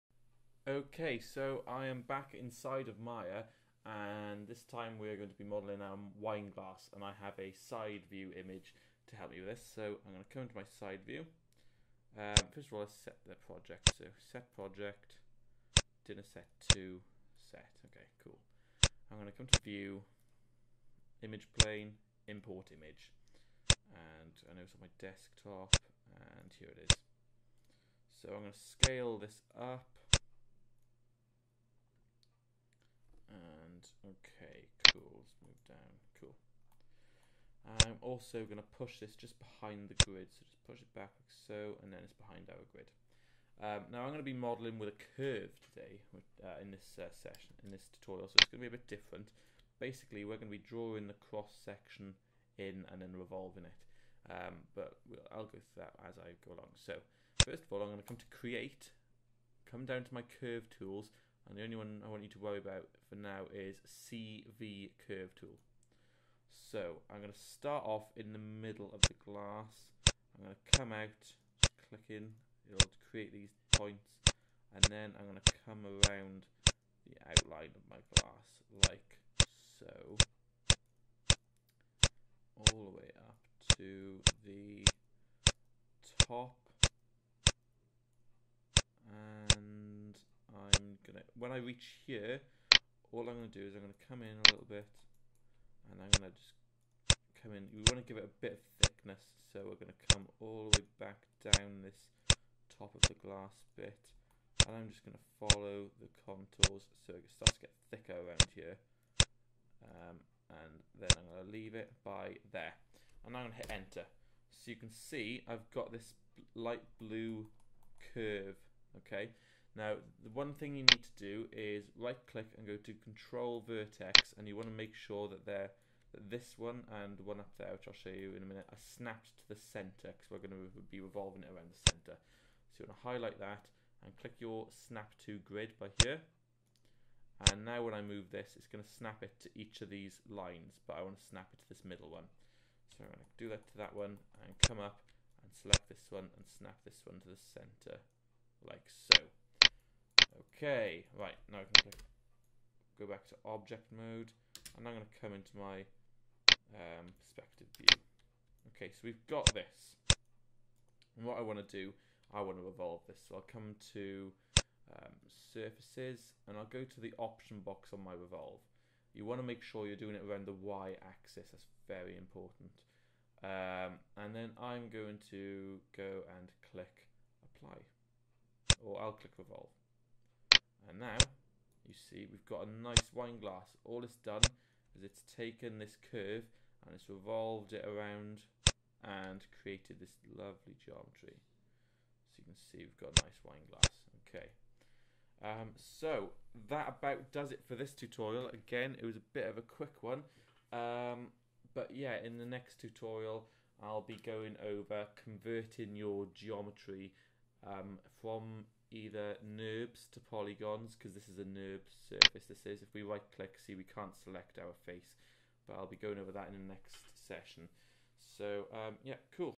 How to model a wine glass inside in Maya
In this tutorial I will show you how to model a Wine Glass in Maya